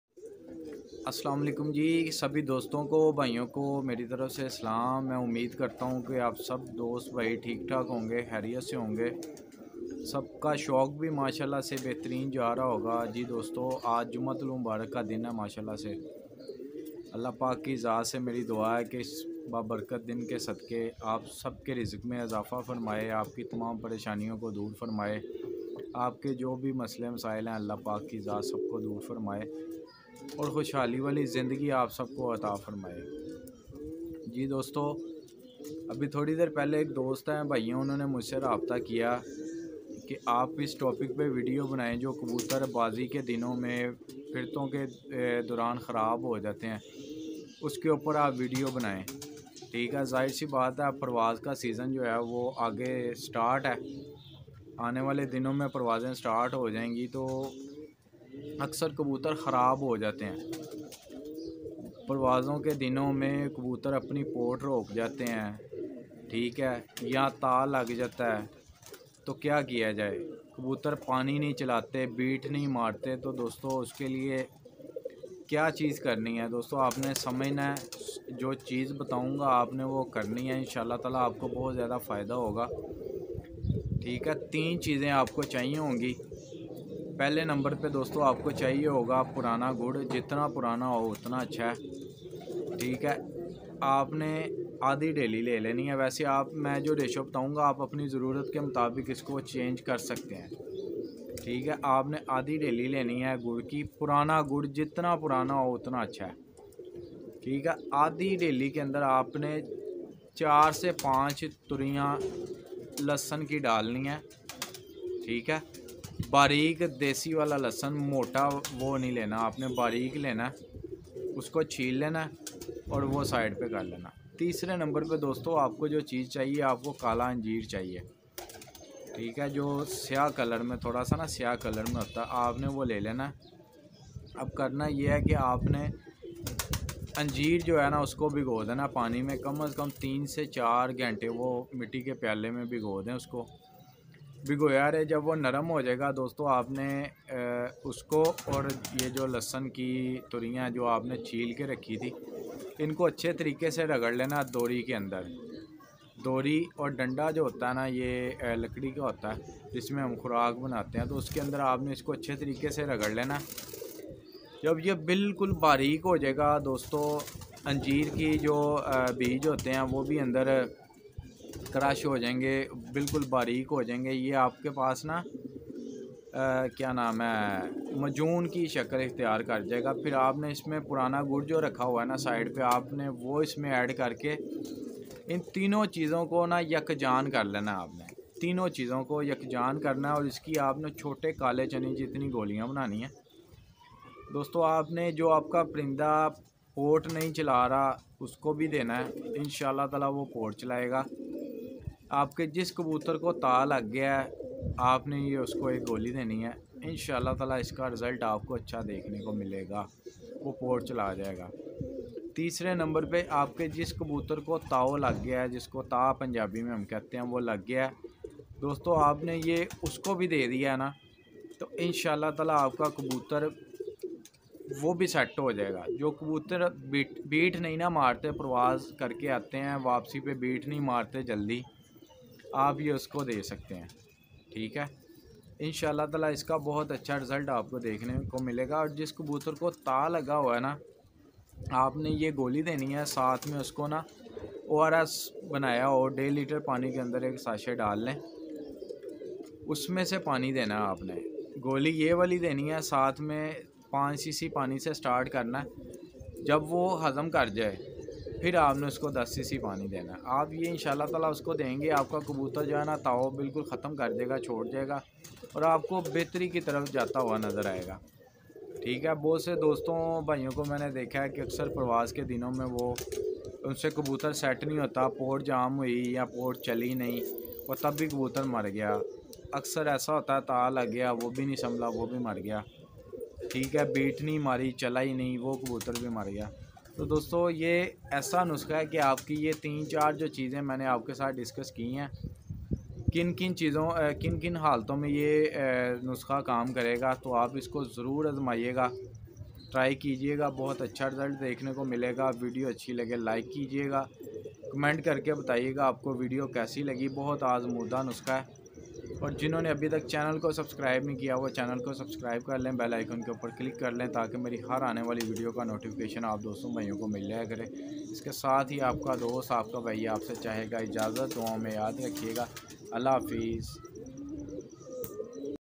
Tao lage kabootar ka ilaj || Jo kabootar pani na chalaye uska ilaj kaise karein || Ustad Malik Asad
Tao lage kabootar ka ilaj
Kabootar pot na chalaye ilaj kaise karein
Kabootar beeth na maray to kaya karein
Parwazi kabootar ko set kaise rakhein
Ustad Malik Asad
Arham Uzair pets vlogs